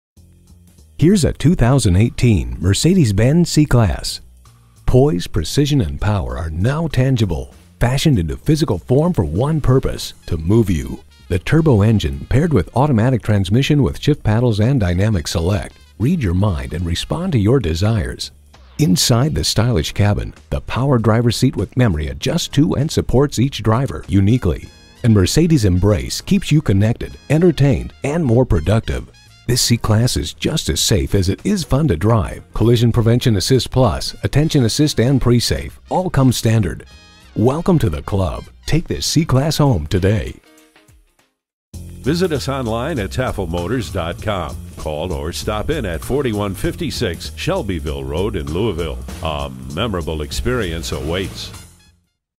2018 Mercedes-Benz C-Class Louisville KY Elizabethtown, KY M17296 - SOLD
Year: 2018
Make: Mercedes-Benz
Model: C-Class
Trim: C 300 4MATIC Cabriolet
Engine: 4 Cyl. 2.0 L
Transmission: Automatic
Color: designo Diamond White Metallic
Interior: Silk Beige/Black
Stock #: M17296
VIN: WDDWK4KB3JF699803

Address:
4156 Shelbyville Road